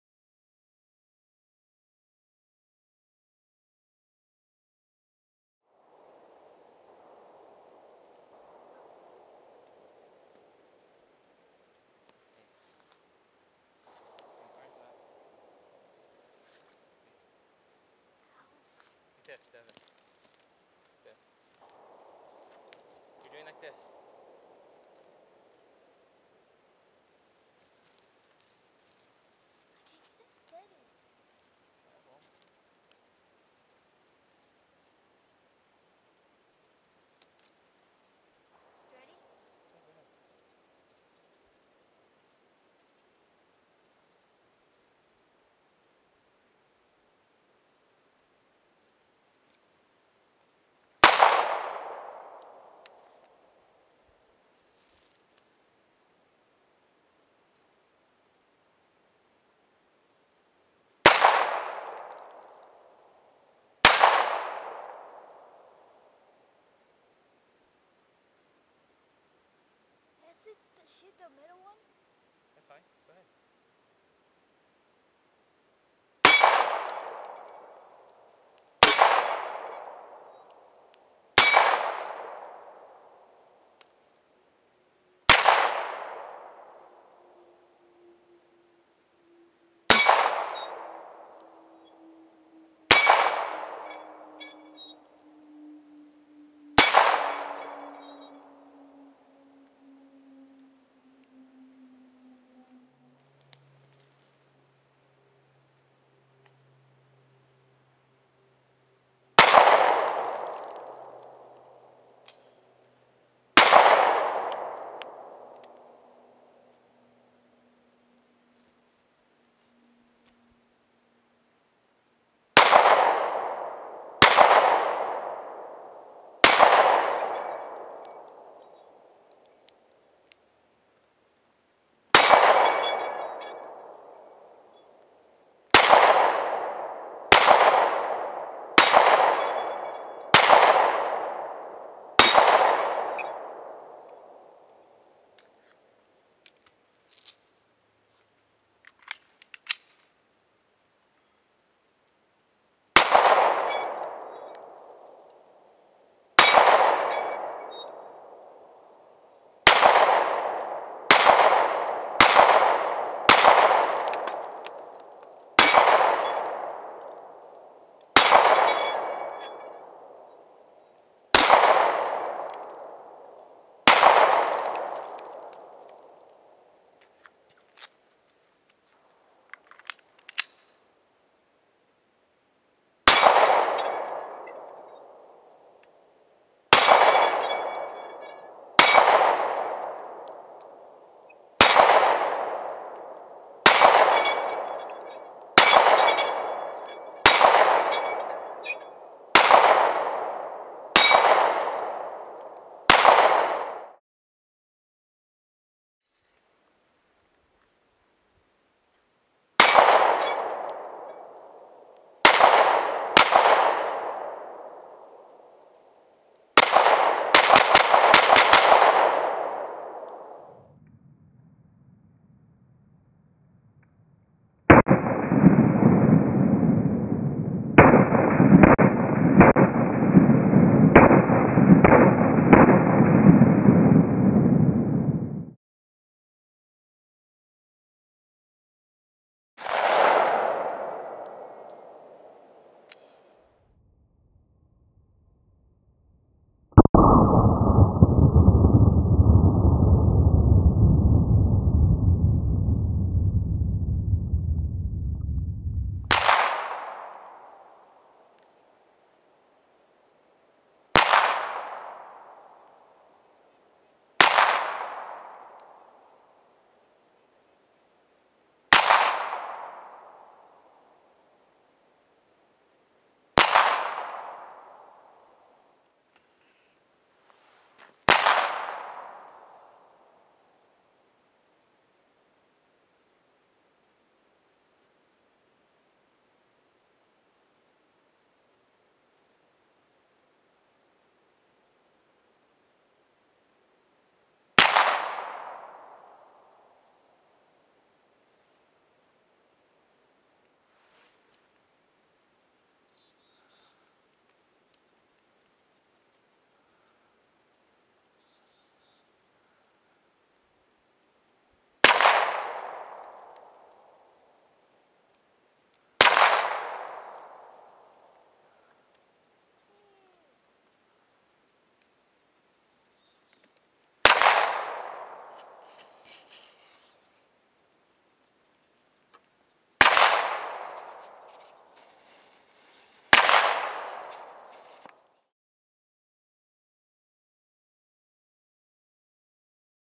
2nd Amendment Day with my Son
On Martin Luther King Jr. Day (2012), I take my son out again to El Dorado National forest for a little 2nd Amendment fun. He also has lessons in history on our Declaration of Independence, Bill of Rights, and how that eventually relates to Martin Luther King Jr.'s vision of the original American Dream realized, "We hold these truths to be self-evident, that all men are created equal."